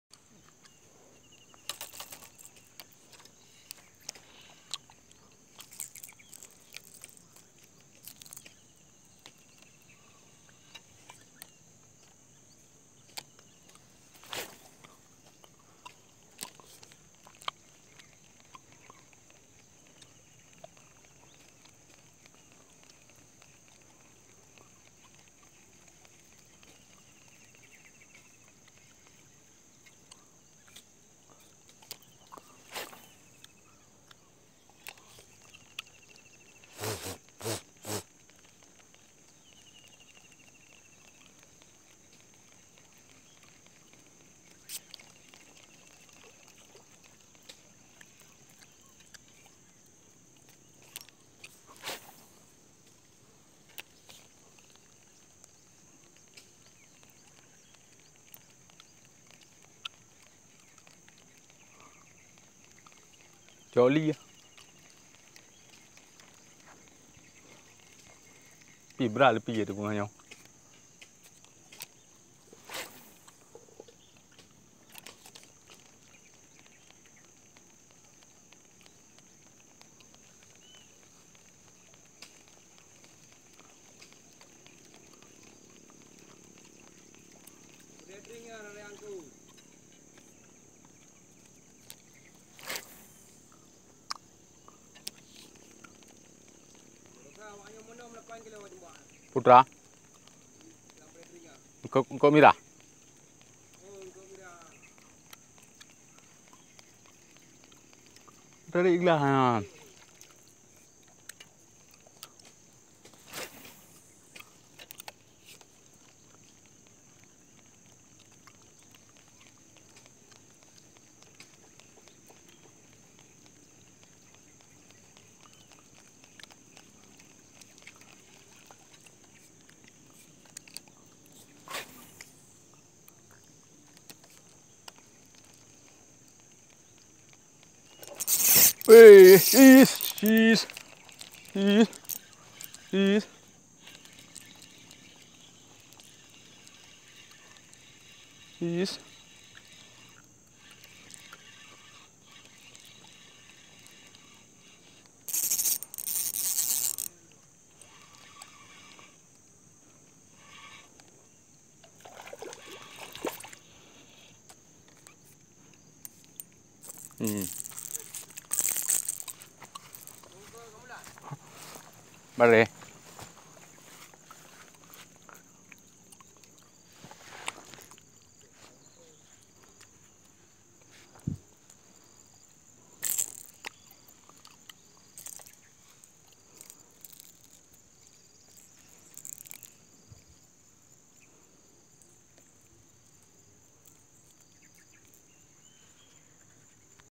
Sambaran hampala dari jauh
Lure amis buat hampala,sambaran pun sangat memuaskan para engler.